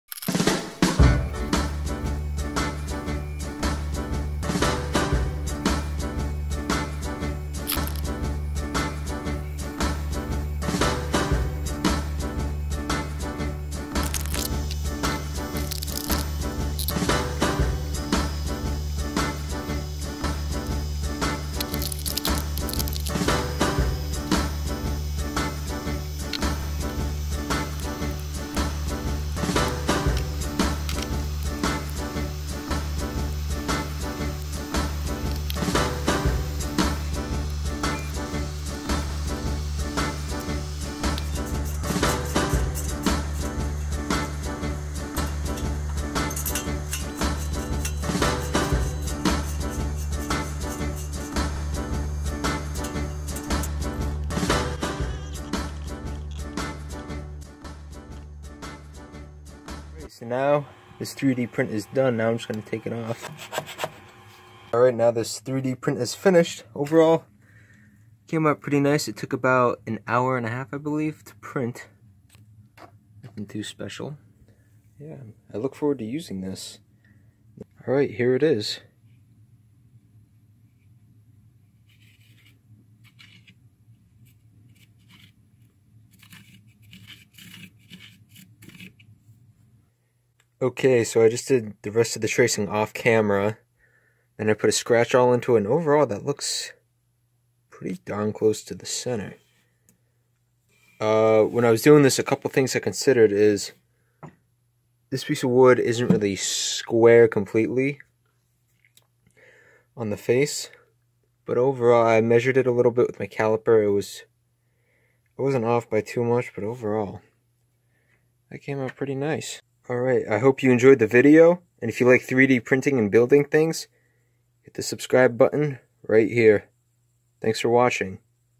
3D Printed Center Finder
This is my 3D Printed Center Finder. This 3D print took about one hour and a half. I will link the STL files and the Ender 3 Printer below.

Creality Ender 3 3D Printer: